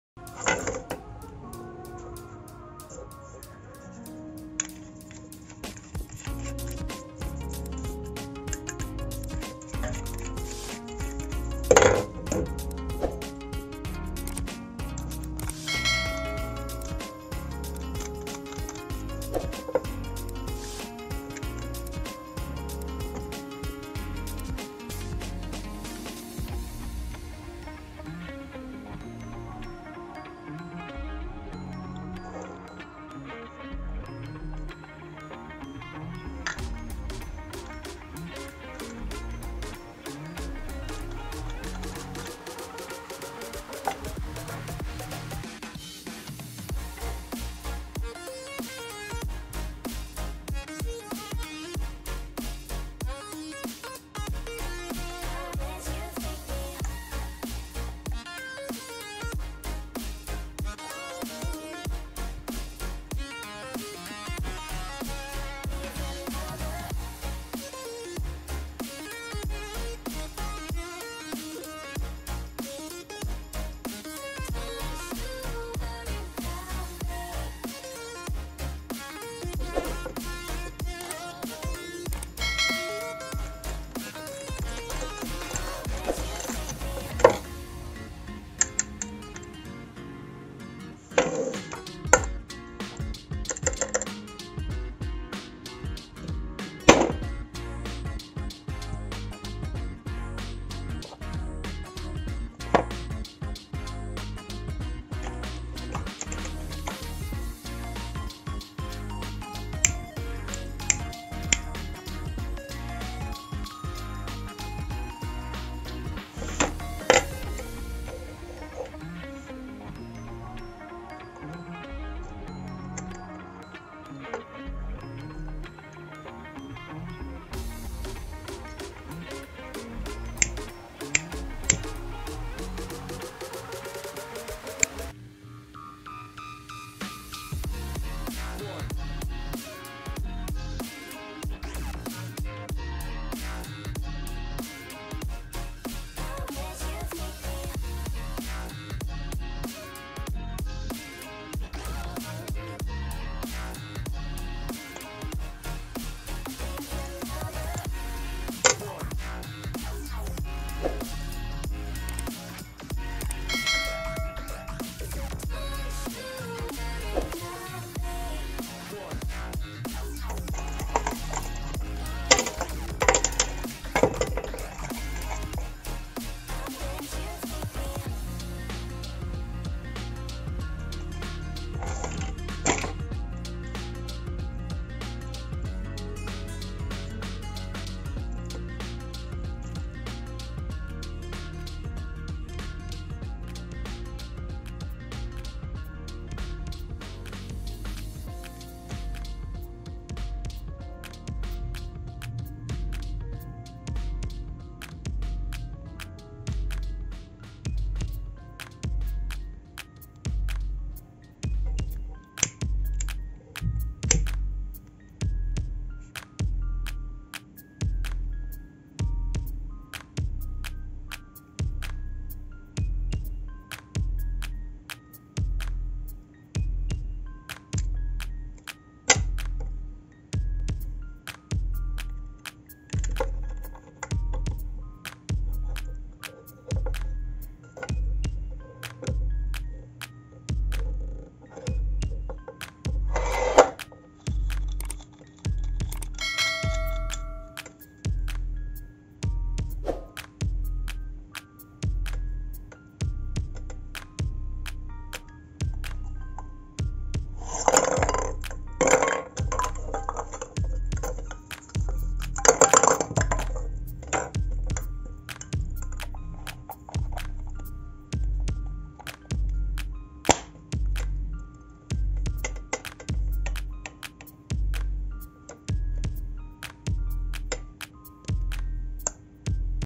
How To Repair The LED Lamp At Home In The Shortest Time/A Fantastic Way To Repair A Light Bulb
With the wonderful method that I taught you in this video, you can repair the LED lamp yourself at home in the shortest time and use it again. Don't miss my videos.